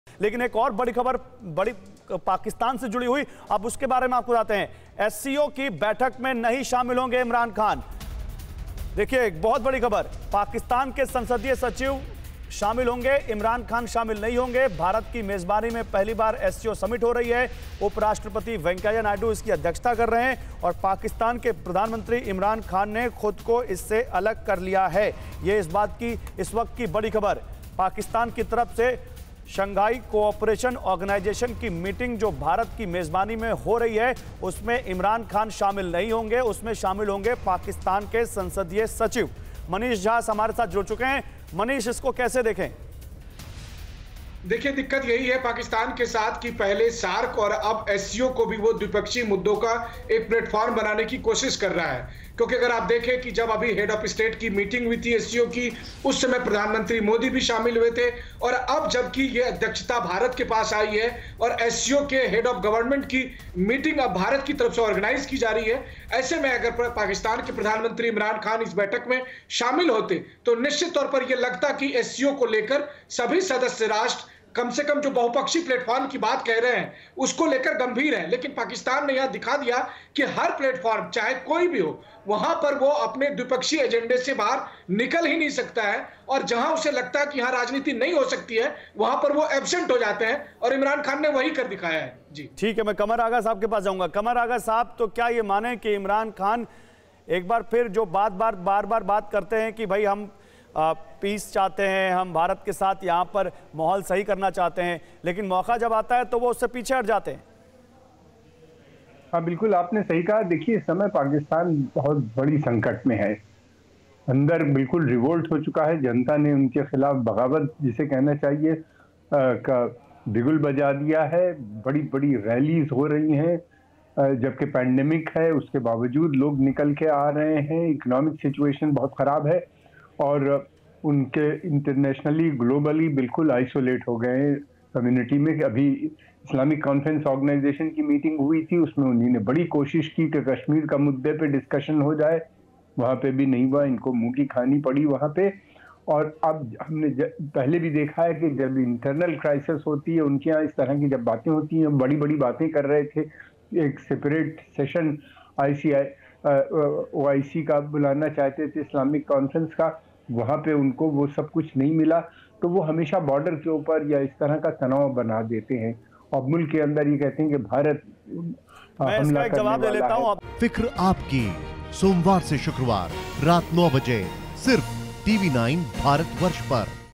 General Bajwa के साथ Imran Khan की बैठक, विदेश मंत्री SM कुरैशी रहे मौजूद
General Bajwa के साथ Imran Khan की बैठक, विदेश मंत्री SM कुरैशी, CJSC चेयरमैन भी रहे मौजूद